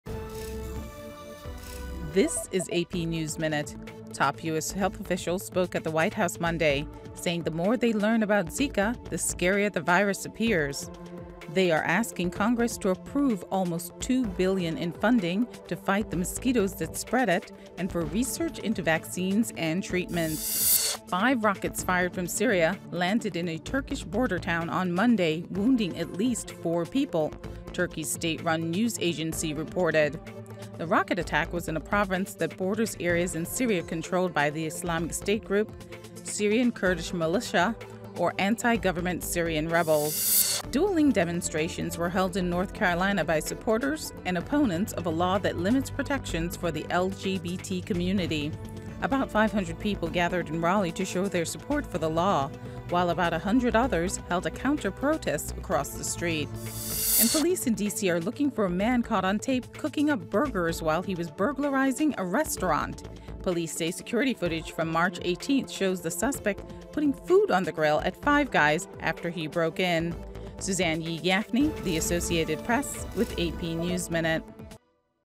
AP Top Stories April 11P
Here's the latest for Monday, April 11th: Health Officials: Zika virus scarier than we thought; Rockets fired from inside Syria land in Turkey; Demonstrations on both sides of NC law; and Burglar in DC cooks during break in.